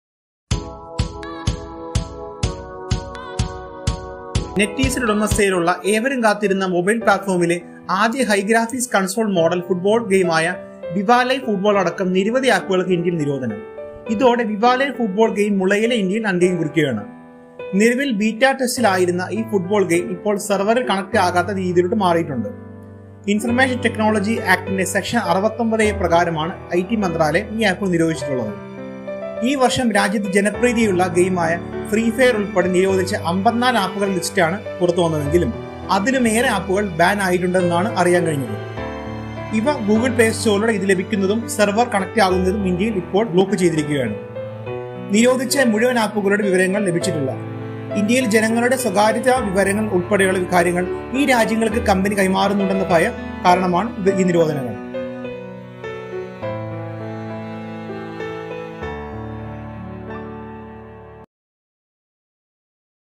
Vive Le Football Banned In India
India Banned Netease games including Vive Le Football , Free Fire Games